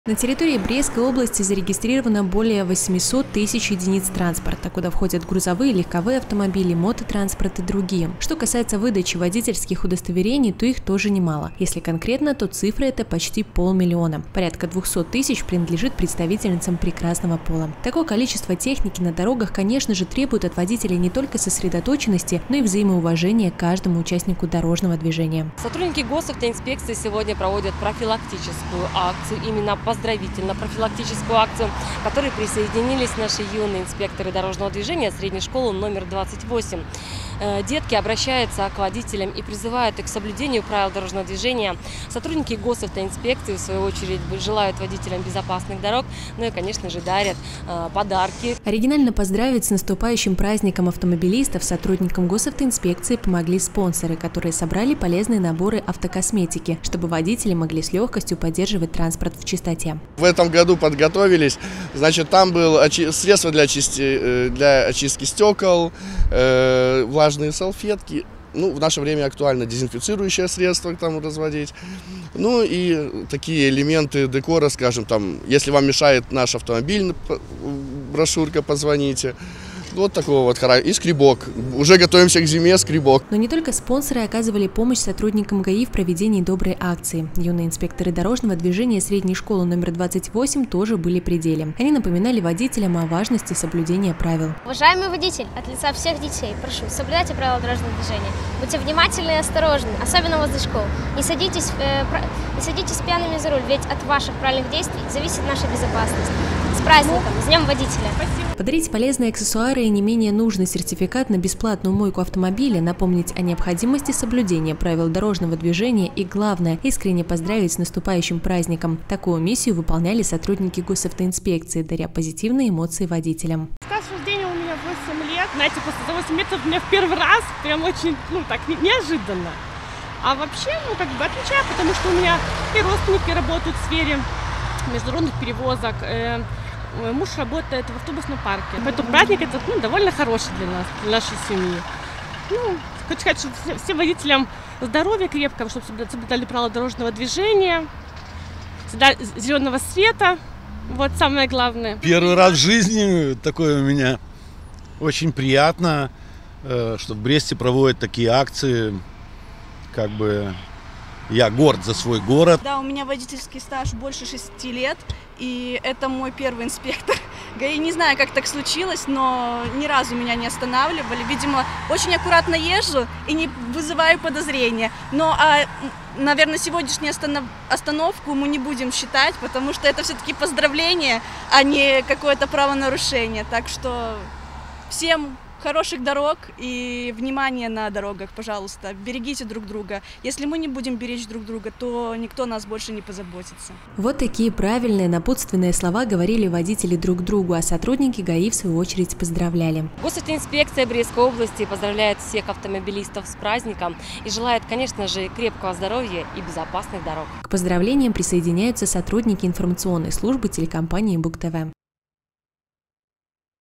2021-10-29 г. Брест. Акция ГАИ ко Дню автомобилиста и дорожника. Новости на Буг-ТВ.
Журналист И. Левчук. Оператор А. Щерба
Уже в это воскресенье   белорусы будут праздновать знаковый день – день автомобилиста и дорожника. Его в нашей стране отмечают ежегодно, начиная с 1998 года. Этот праздник стал всенародным признанием заслуг транспортников в развитии экономики Беларуси. Сегодня, специально ко дню автомобилиста и дорожника, в областном центре сотрудники Госавтоинспекции провели поздравительно-профилактическую акцию. Наша съемочная группа присоединилась к доброй инициативе. Все подробности далее.